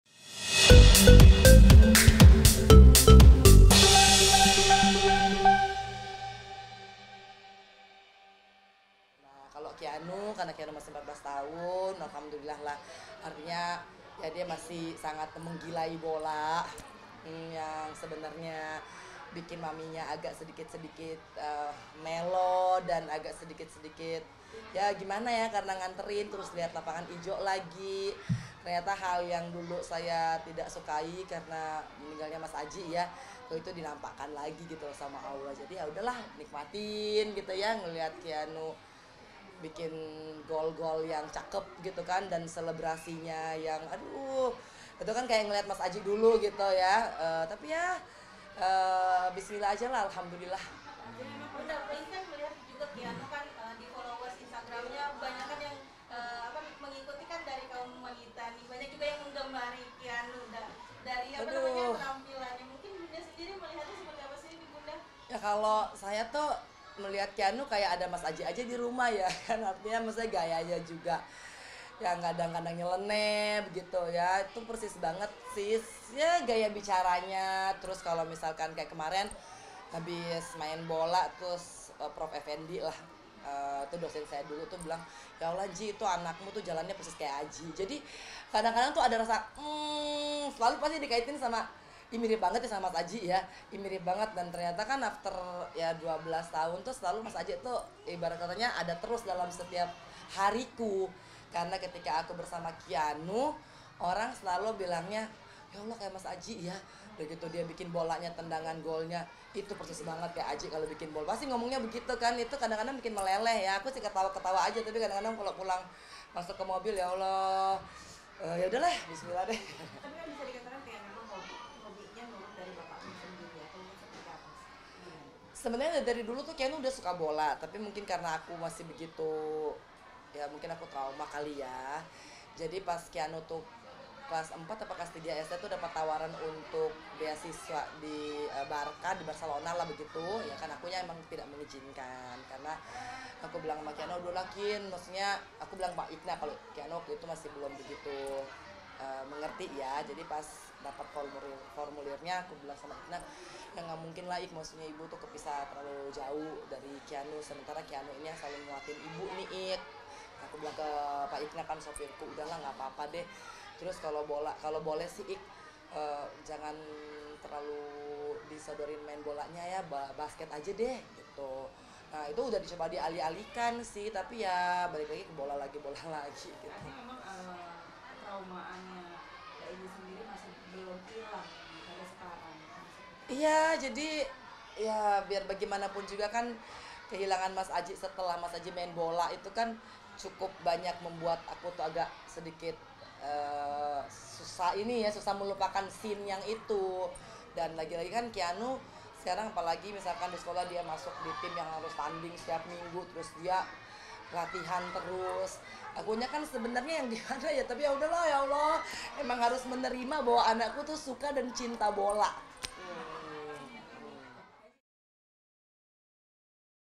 ANGELINA SONDAKH LARANG PUTRANYA MAIN SEPAK BOLA, ADA APA?
Diketahui bahwa mendiang Adjie Massaid tutup usia karena serangan jantung usai main bola karena hal tersbut Angelina Sondakh tak pernah mengizinkan putranya bermain bola, namun hal tersbut akhirnyta dipasrahkan dan diterima oleh Angelina Sondakh dengan lapang dada.